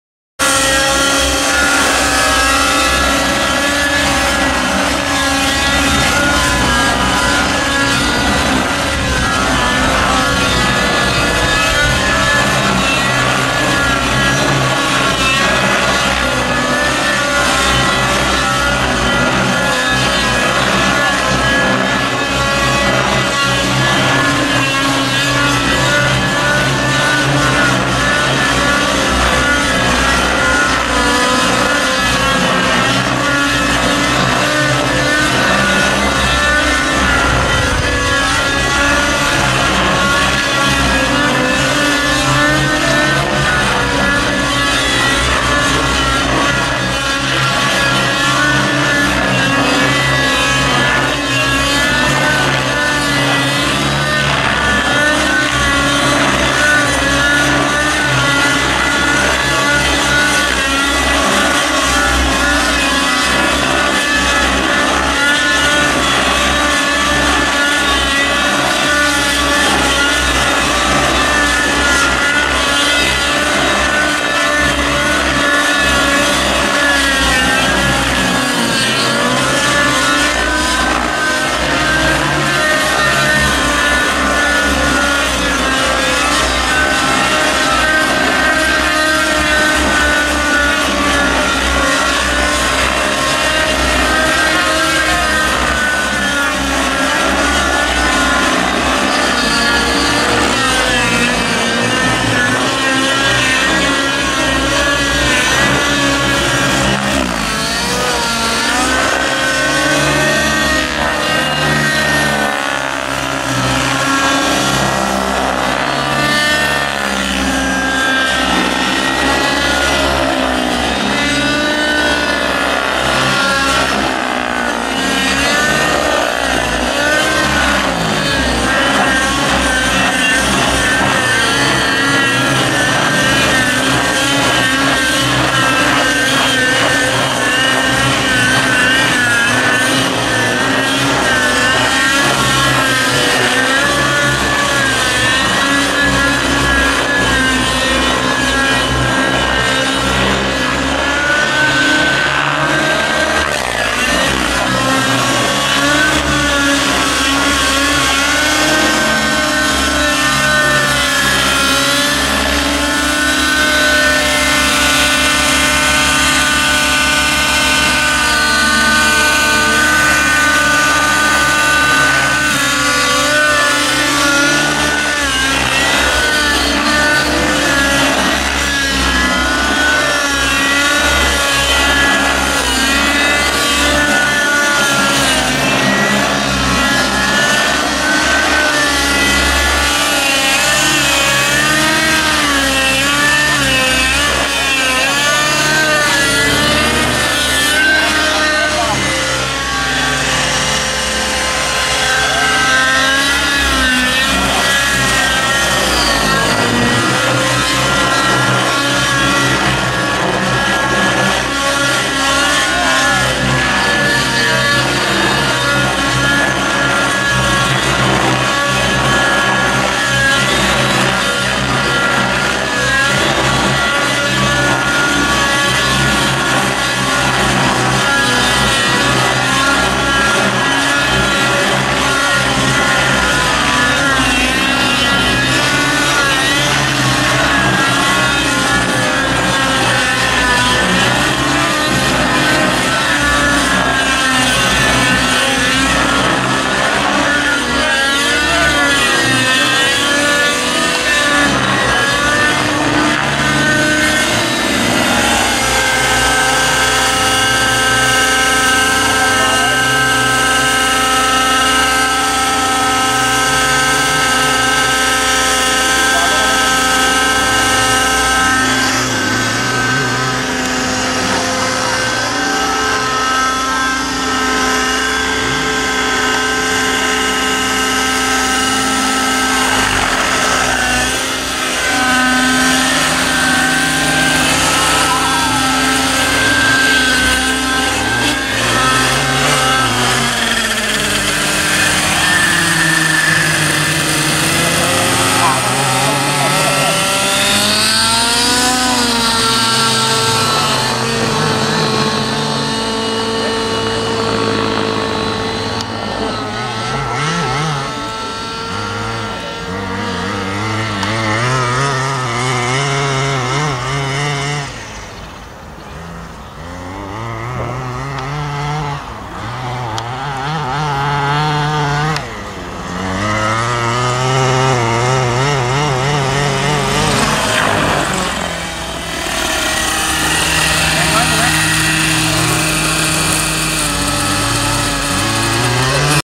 บีม กับ หนี่ง บินคู่ให้ฝรั่งดูเพลินๆ
บีม กับ หนื่งในวันพักผ่อนหลังการแข่งขัน 3D Master